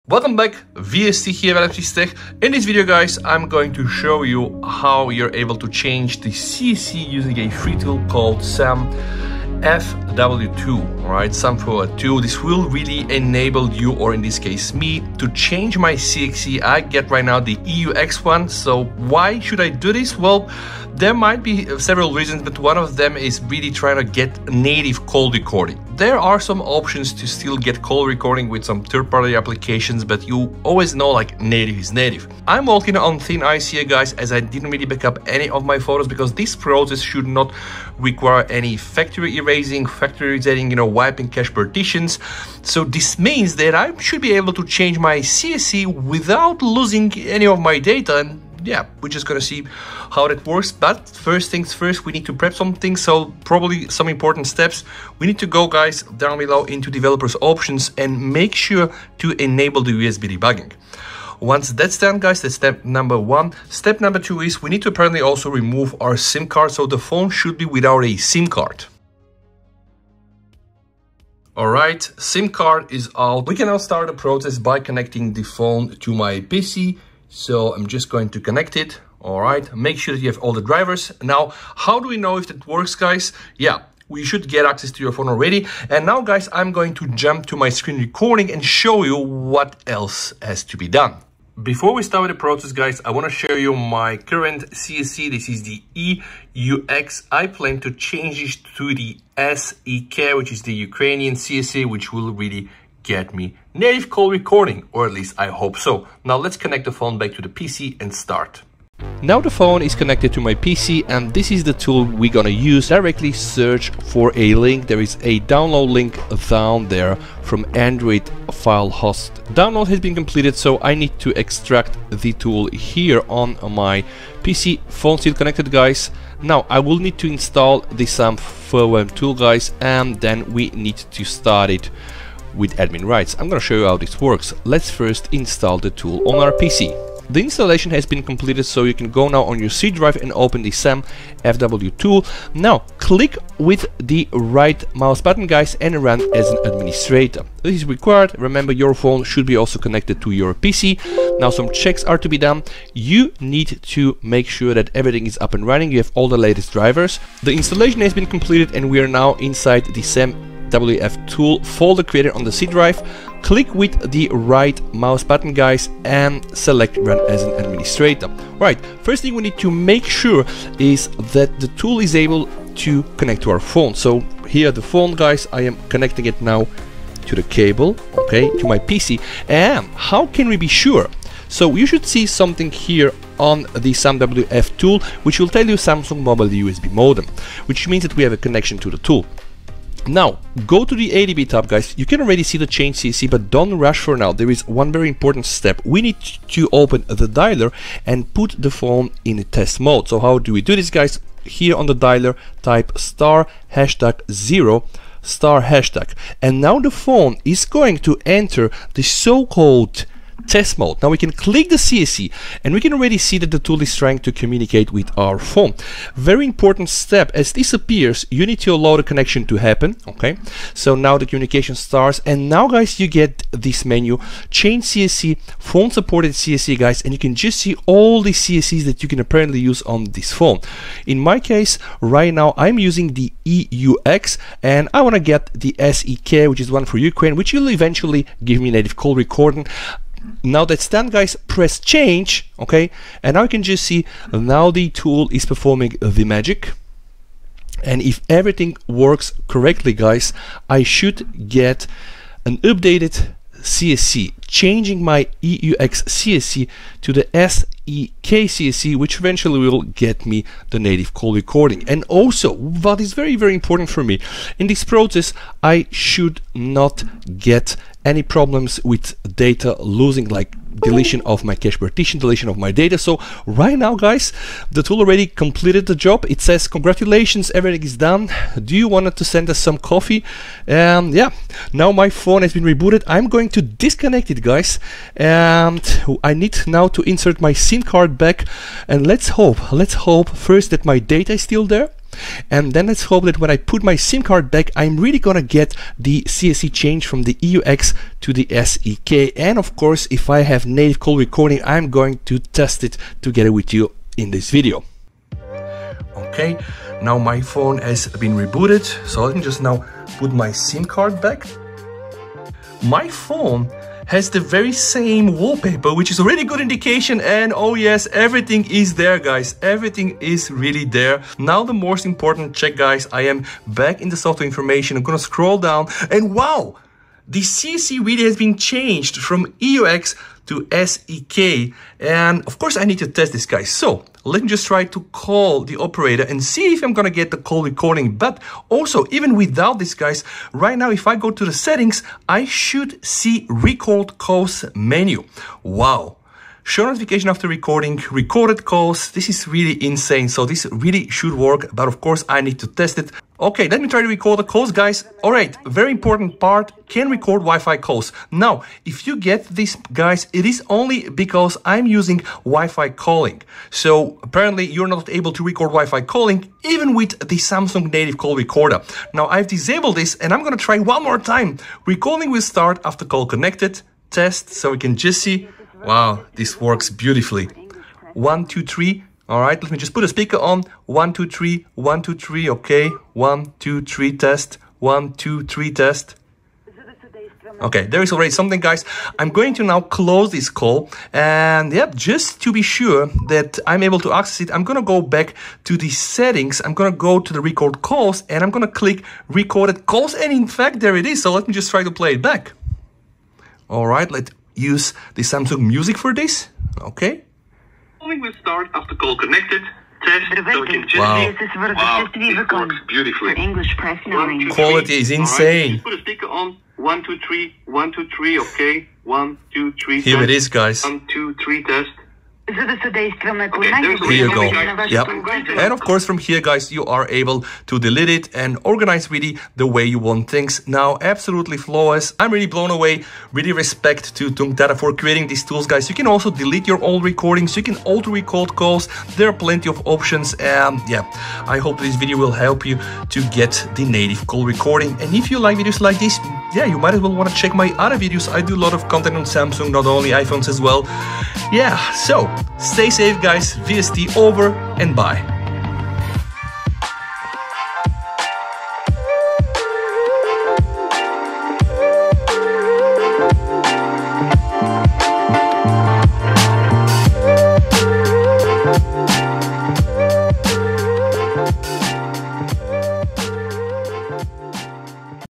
Enable NATIVE CALL Recording on your Galaxy S23 Ultra in 5 minutes!
▶ CSC codes with working Call Rec option:
SEK, INU, INS, XXV, THL, ILO, EGY, BNG, CAM, MYM, LAO, LYS, NPL, TMC, WTL, TUN - depends of your phone model
SEK - Working call recorder, Missing SPay, Missing CallID
INS -  Working call recorder, Working SPay, Working CallID - Missing OTA if you are coming from Exynos (B) version, if you are coming from E version you will have OTA
XXV- Working call recorder, Working SPay, Missing CallID
THL - Working call recorder, Working SPay, Working CallID
INU - Working call recorder, Working SPay, Working CallID